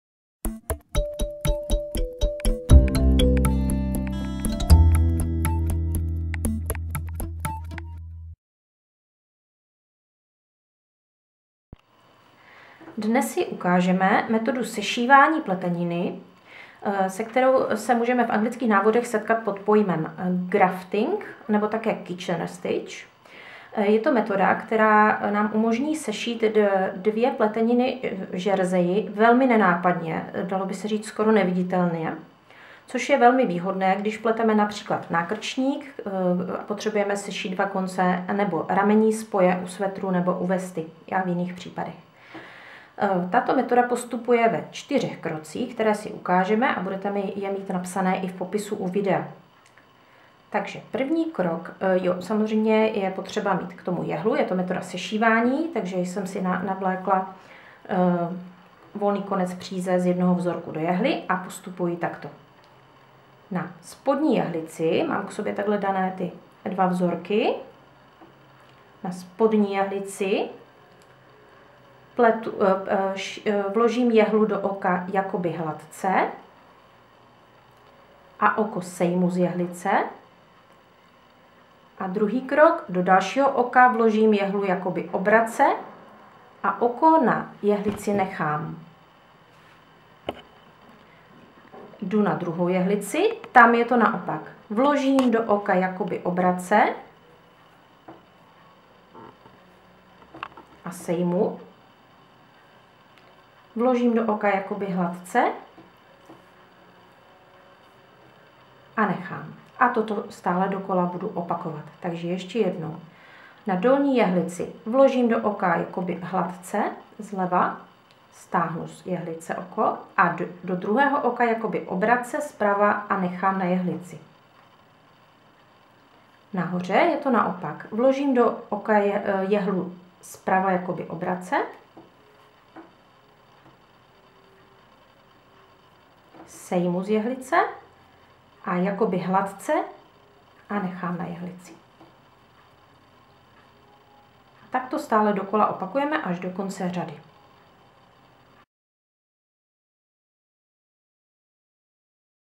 Pletařské techniky - sešívání žerzeje (tzv. grafting)
Postupujeme takto:
Oba kousky pleteniny, které chceme sešívat si zorientujeme lícem nahoru a jehlicemi k sobě. Do jehly si navlékneme přízi.
*1) na dolní jehlici vložím jehlu do oka jakoby hladce, sejmu oko, do druhého oka na této jehlici vložím jehlu jakoby obrace, oko nechám na jehlici.
2) na horní jehlici vložím jehlu do oka jakoby obrace, sejmu oko, do druhého oka na této jehlcii vložím jehlu jakoby hladce, oko nechám na jehlici* opakuji až do konce.